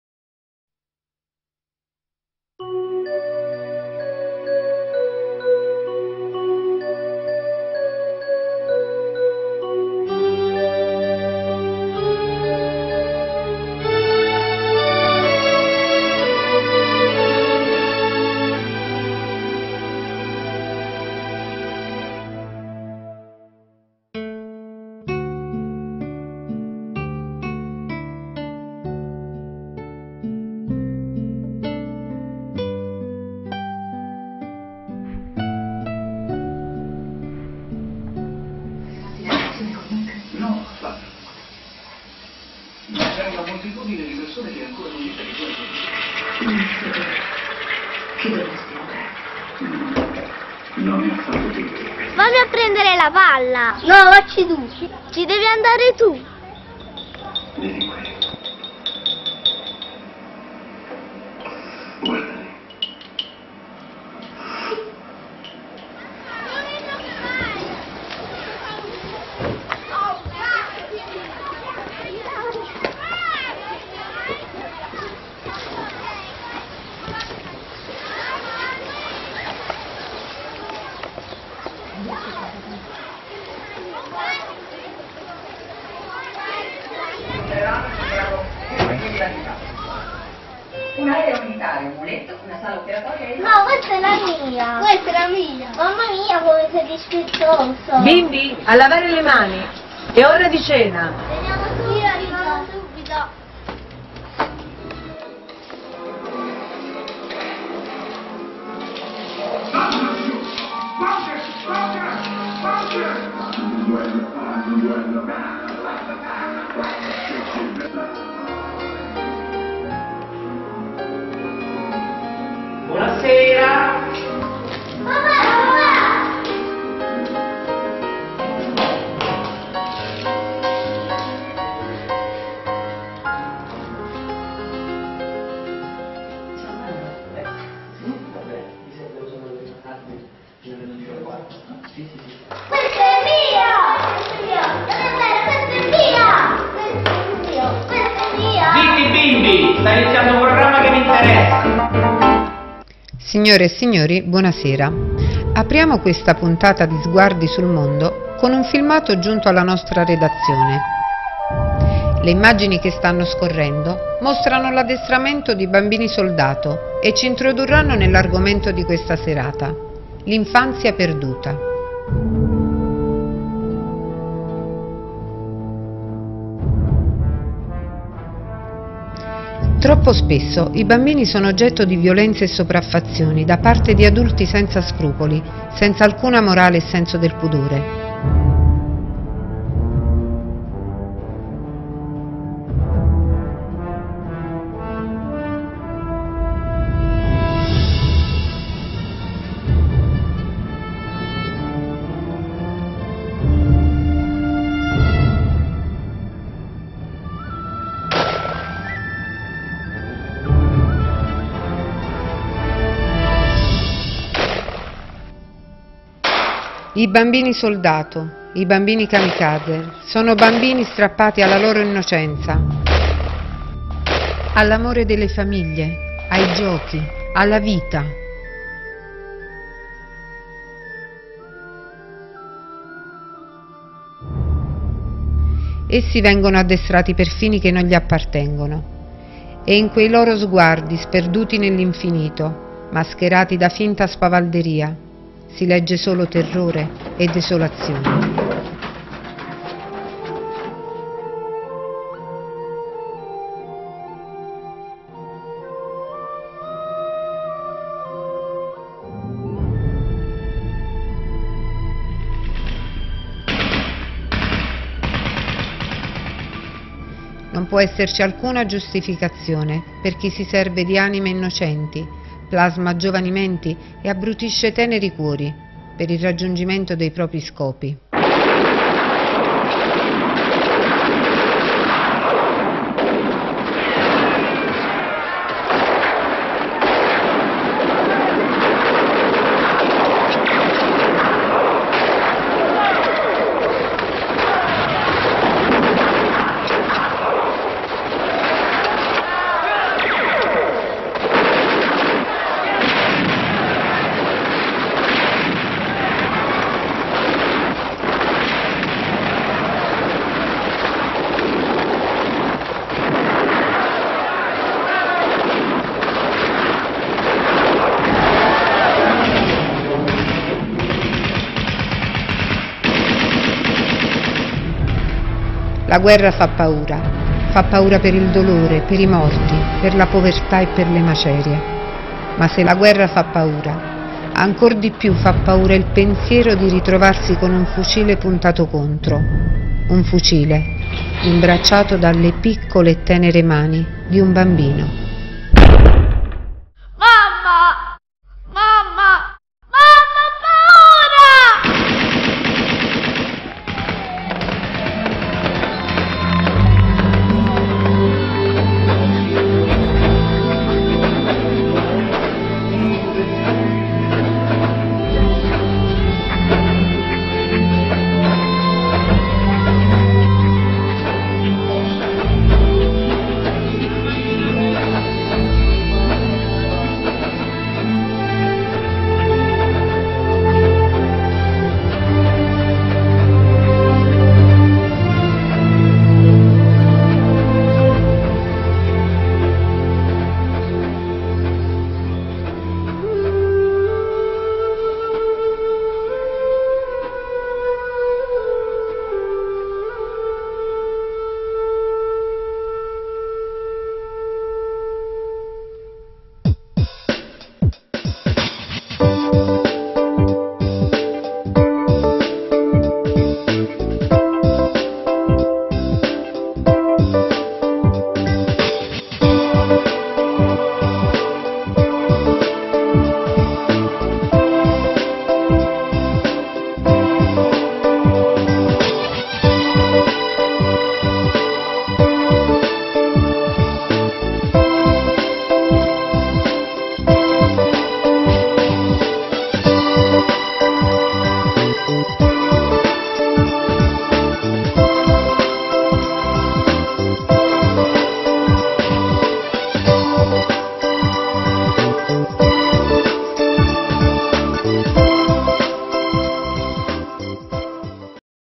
'Lasciateci giocare'
'Lasciateci giocare' - I Classificato nella sezione 'Interculturalità e Pace' della I ed. dei Bandi di Idee

Trama:

Una famiglia tranquilla... la vita scorre normalmente, serenamente... due bambini giocano spensierati e felici...giocano come ogni bambino al mondo dovrebbe giocare.
Di sera, come al solito, tutti riuniti intorno alla TV..., ma qualcosa turba gli animi di grandi e piccini.